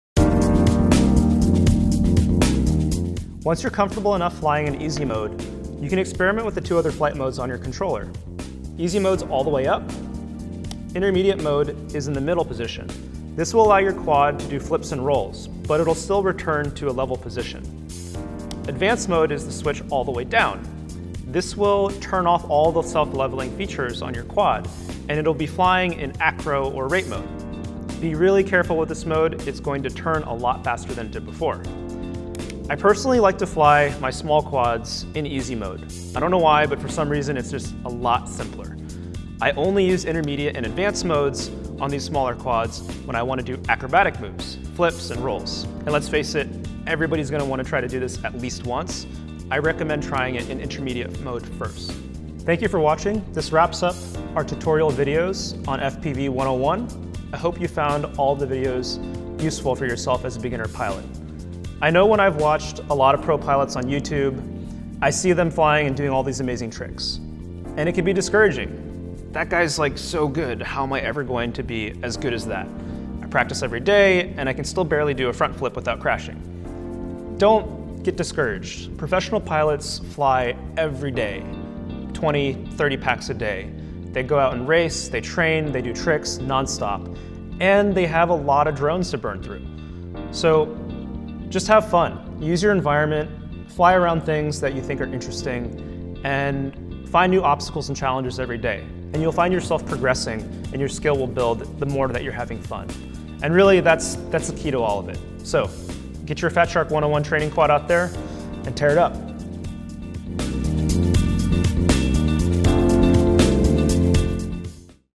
Fat Shark 101 Training, Episode 9: Advanced FPV Flight
IN THIS EPISODE: How to fly full scale FPV and use the advanced modes in the Fat Shark 101 Drone Training Kit